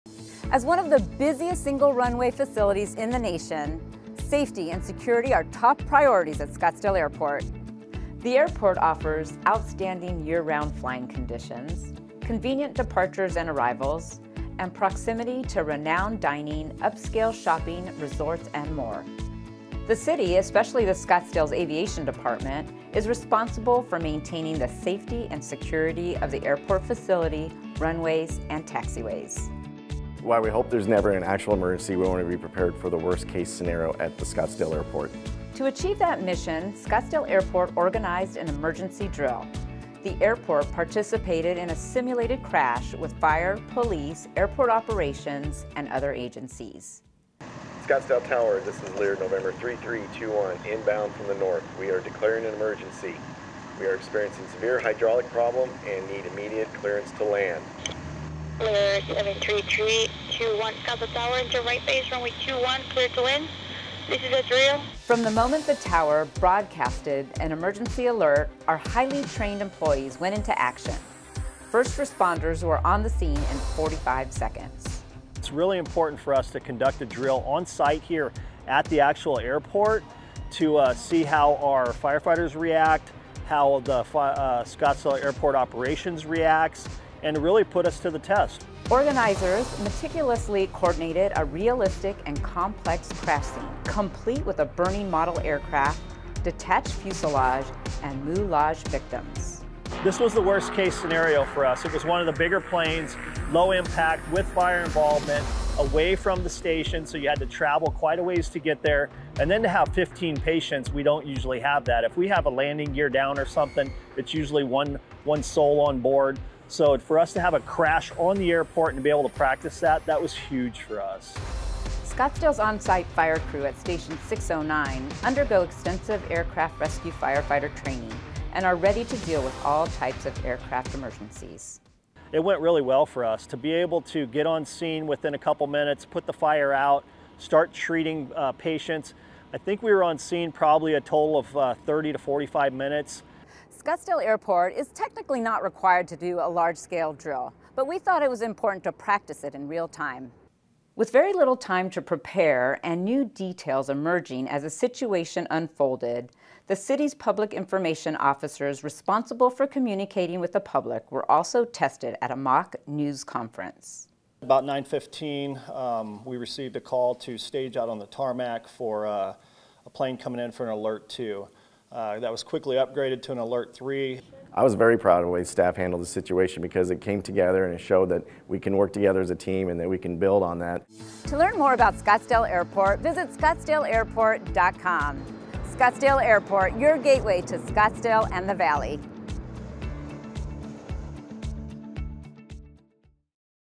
Scottsdale Airport Emergency Response Drill
Watch as the Scottsdale Airport’s simulated emergency response drill unfolds, where aviation staff, police and fire departments come together to test their preparedness in a high-pressure scenario. With a prototype Lear Jet engulfed in flames, moulage victims and a well-crafted script, this drill was designed to simulate a real-life emergency, challenging staff to react quickly and effectively.